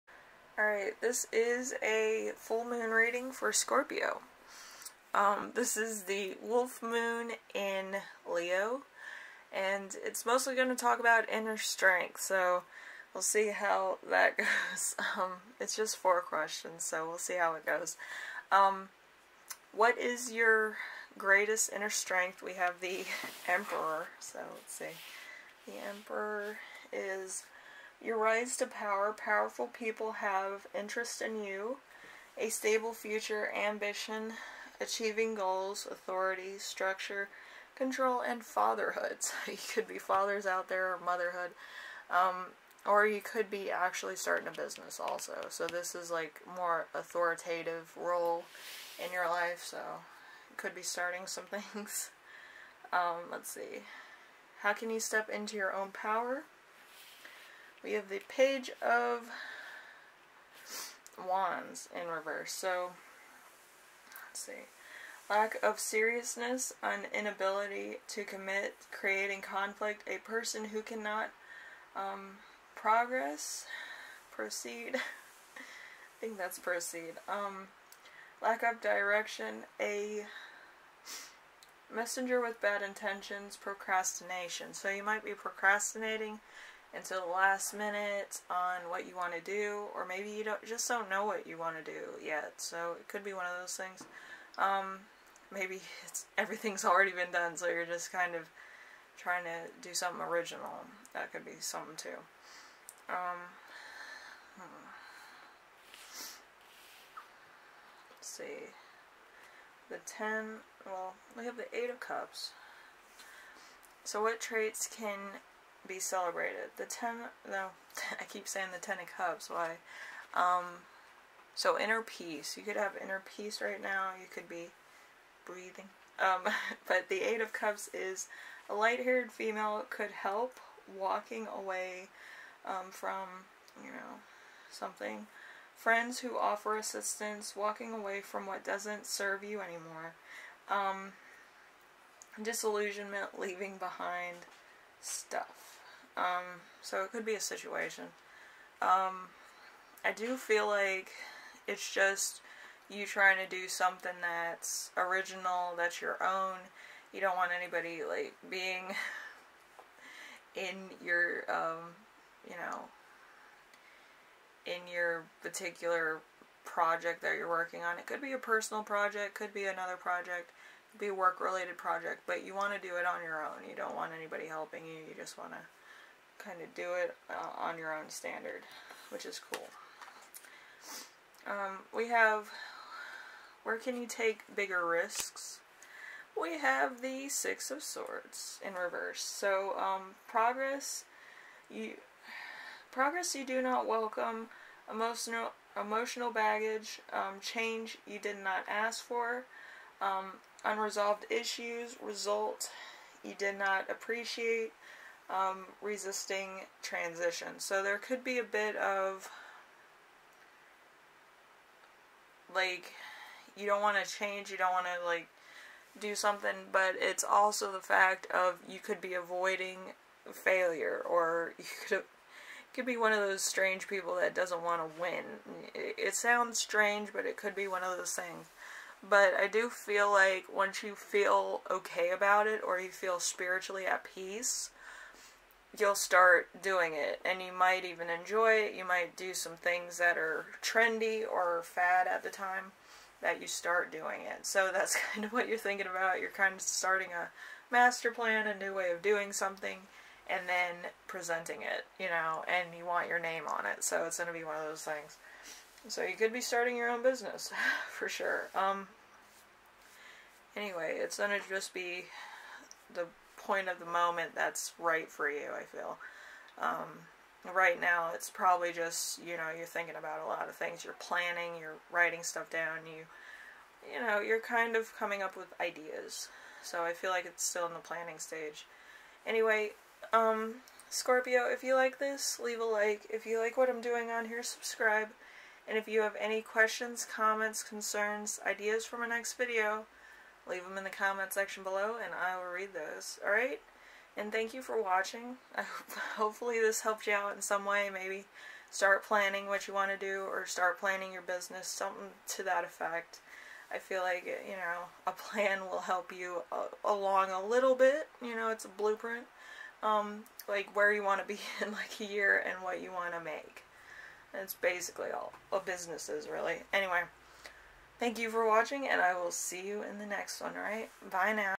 Scorpio January Wolf full moon in Leo January 28, 2021.
1.What in my greatest inner strength?

2.How can I step into my own power?

3.What traits can be more celebrated?

4.Where can I take bigger risks?